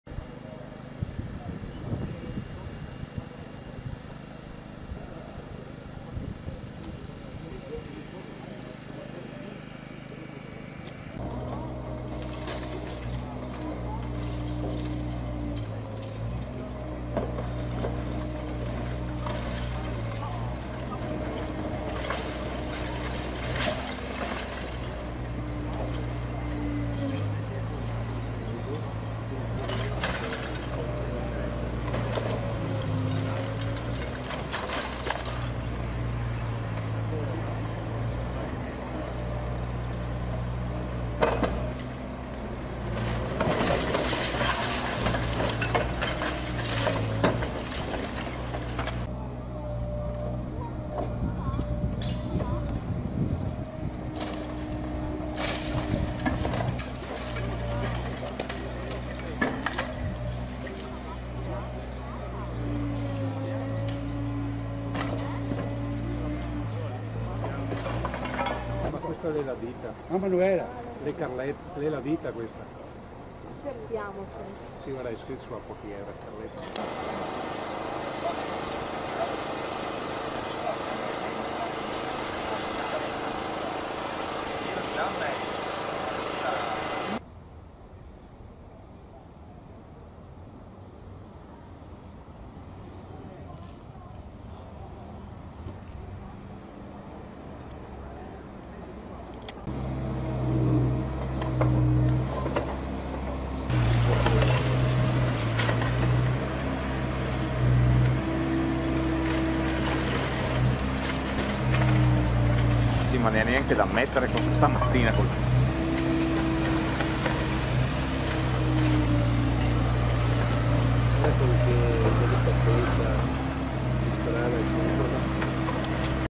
Brugnera, una pagina di storia se ne va: abbattuta casa Bazzo
A Brugnera è stata demolita casa Bazzo, l'edificio al civico 13 di via Vittorio Veneto dichiarato inagibile nel dicembre 2017 scorso e a rischio crollo dal 22 febbraio. La casa, giorno dopo giorno, è sprofondata sotto terra: per quasi cento anni la porta di ingresso è stata a livello strada mentre prima di essere demolita era al di sotto del piano stradale di oltre 50 centimetri (video Missinato).